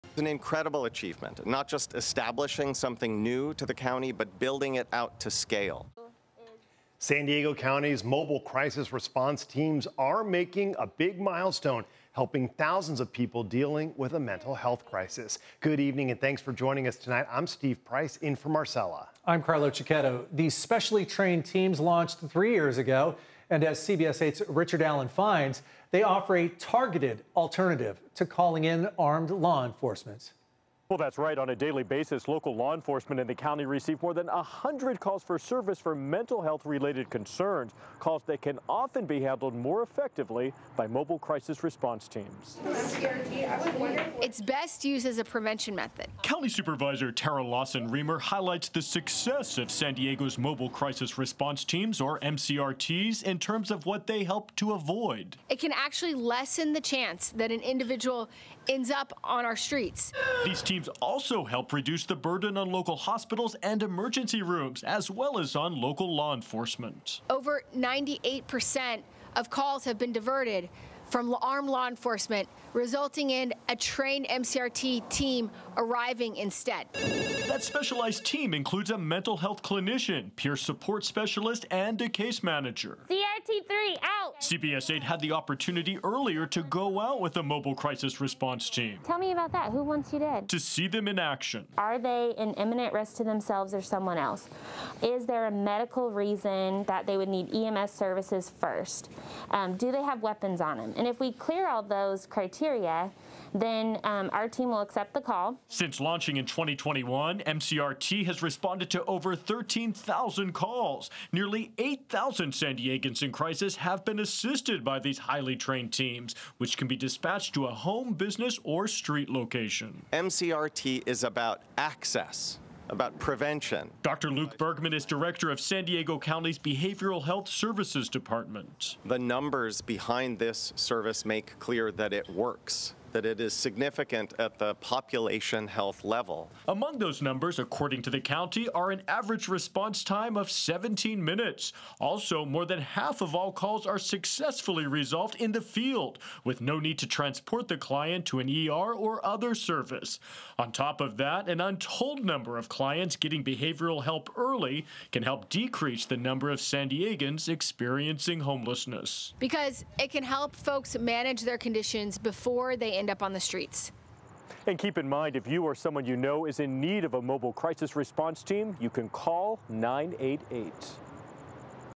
Mobile crisis response teams helping thousands of San Diegans
These teams help reduce the burden on local emergency rooms and law enforcement, and can help prevent San Diegans from falling into homelessness.